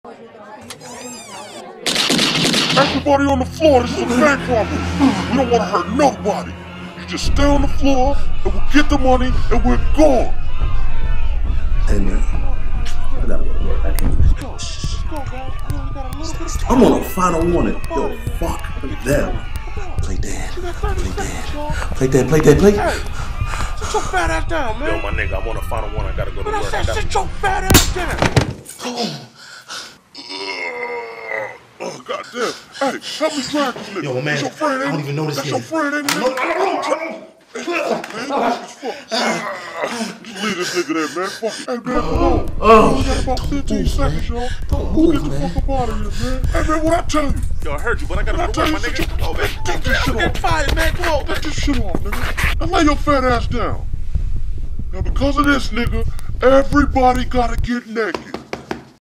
Bank Robbery (Reedo X Woody)
Reedo Brown X Woody The Great got in to a bit of a situation at the bank.